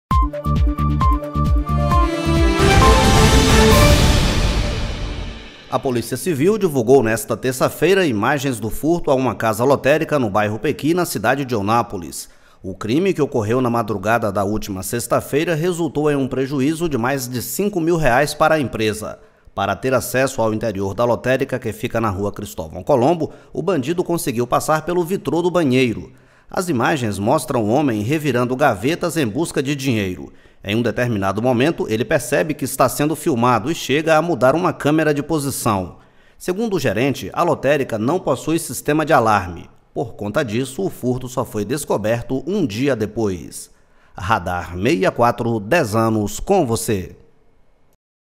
Homem invade lotérica e furta mais de R$ 5 mil; Câmeras registram crime
Ao ver que estava sendo filmado, ele tentou mudar câmera de posição